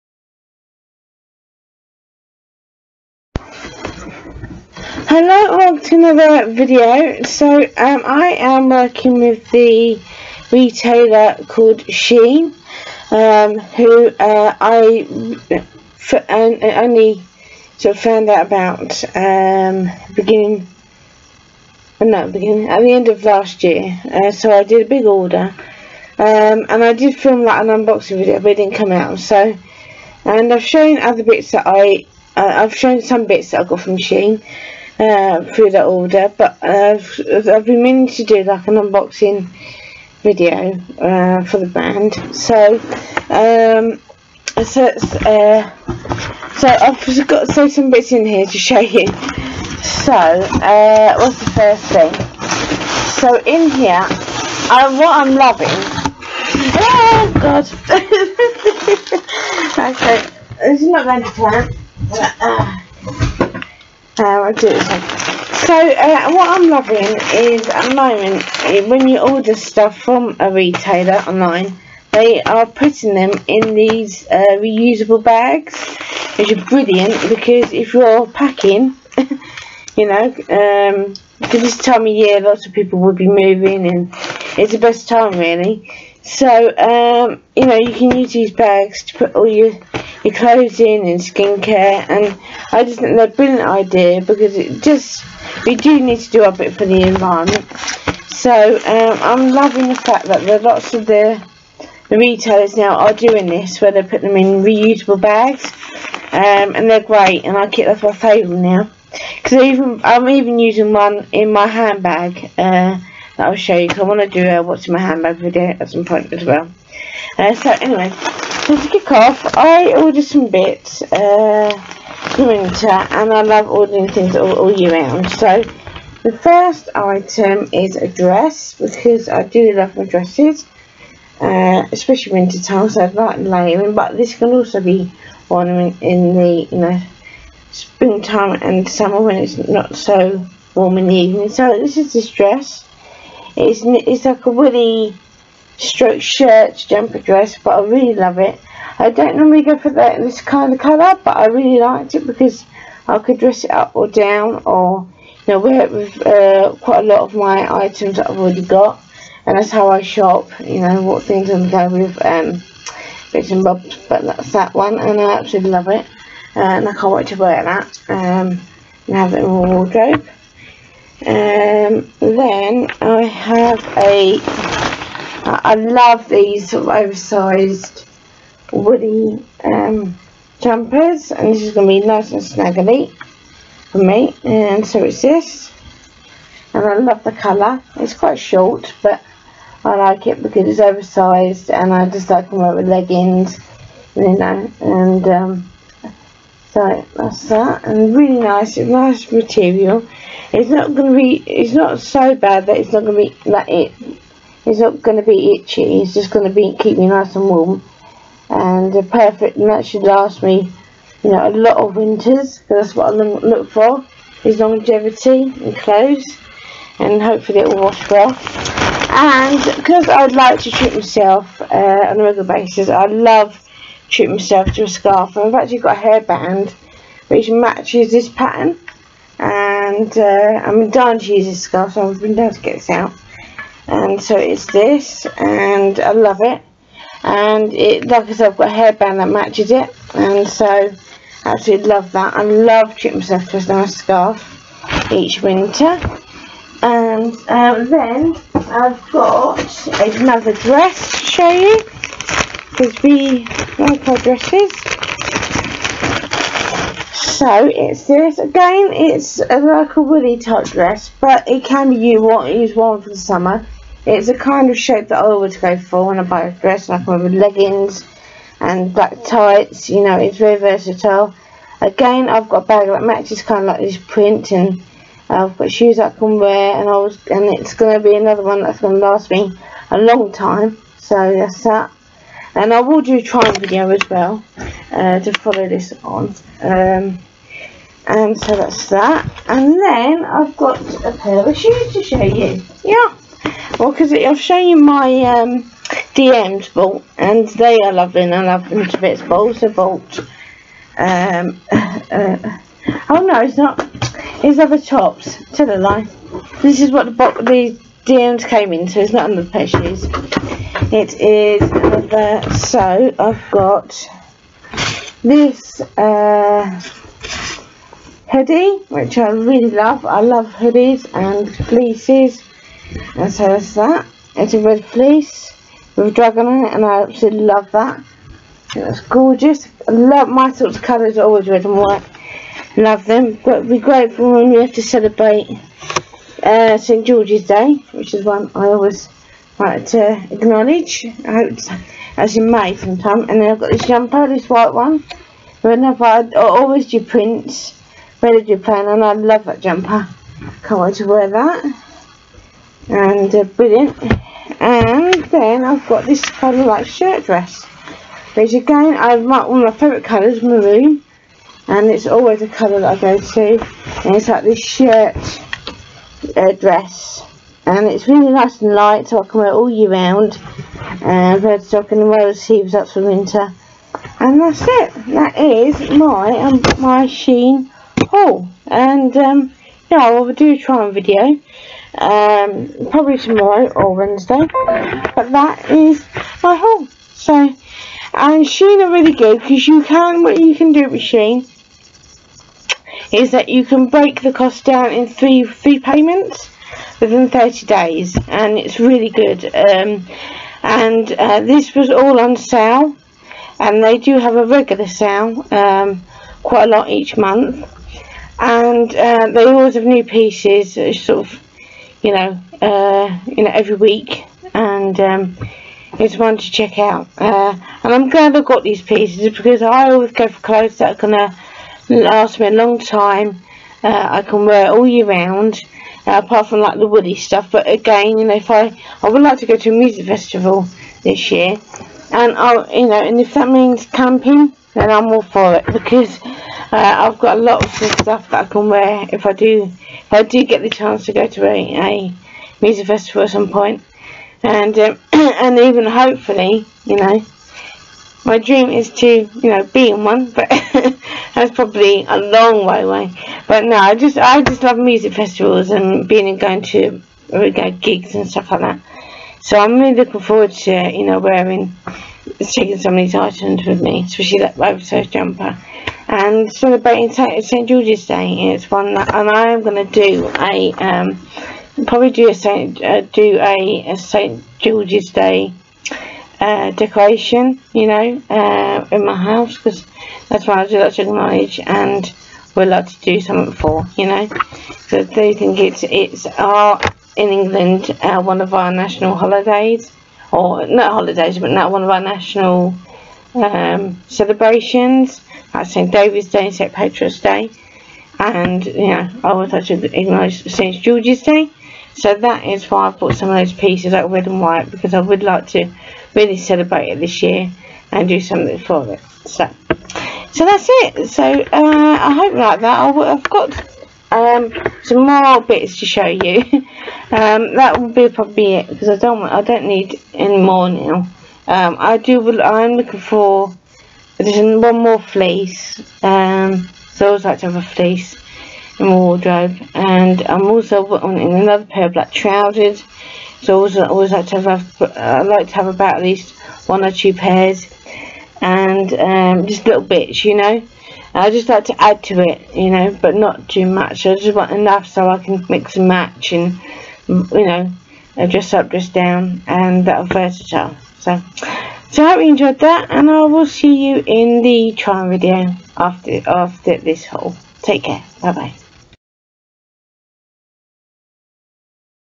AD Shein Haul video/Part 1 as a try on video will follow
Hello and welcome to another Haul video, this time it is items all bought from Shein. All these videos will be uploaded to my Squarespace